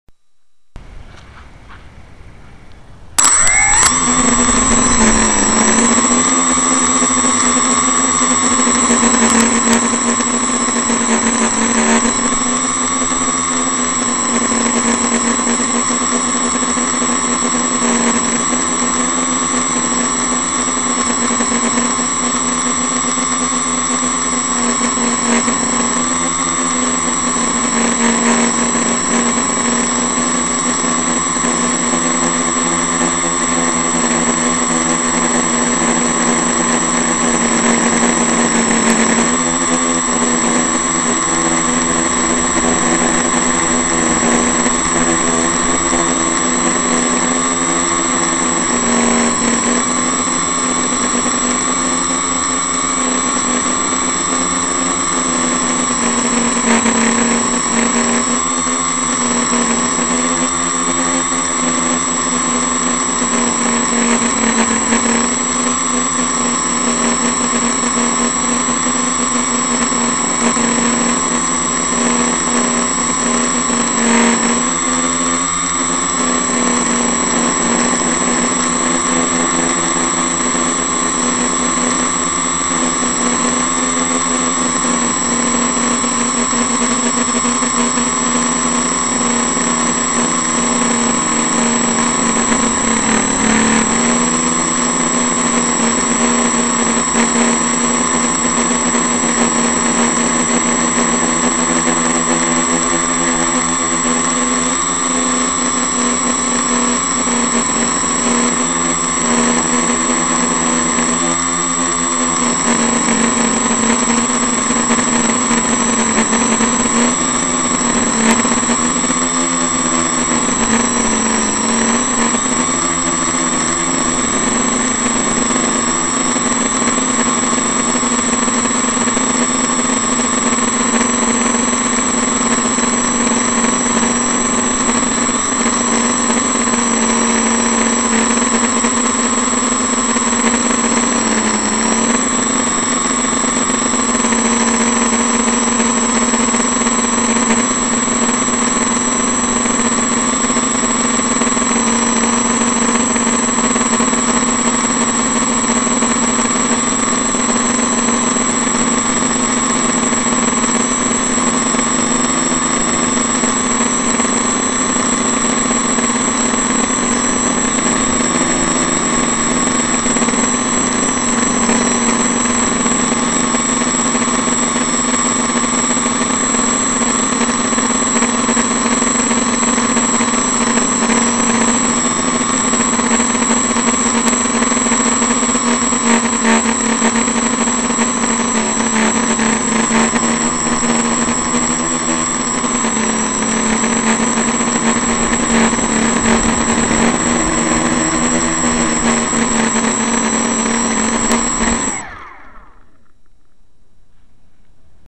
My OP CC on a little windy day, hovering in my backyard
Still playing with Kp, Ki and other settings on my OpenPilot CC. This is improvement over the default settings. More stable, but it was quite windy, so the stability is not that visible. I have to go to an open field to try more stuff.